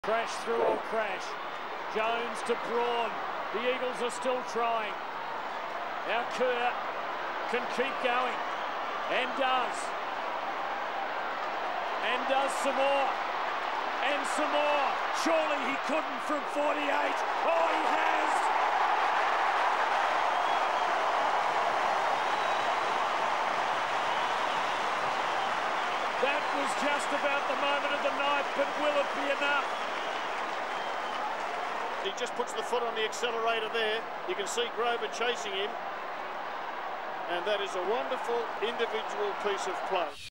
Daniel Kerr 2003 Goal Of The Year
West Coast Eagles Daniel Kerr kicks the official 2003 AFL Goal of the Year against Fremantle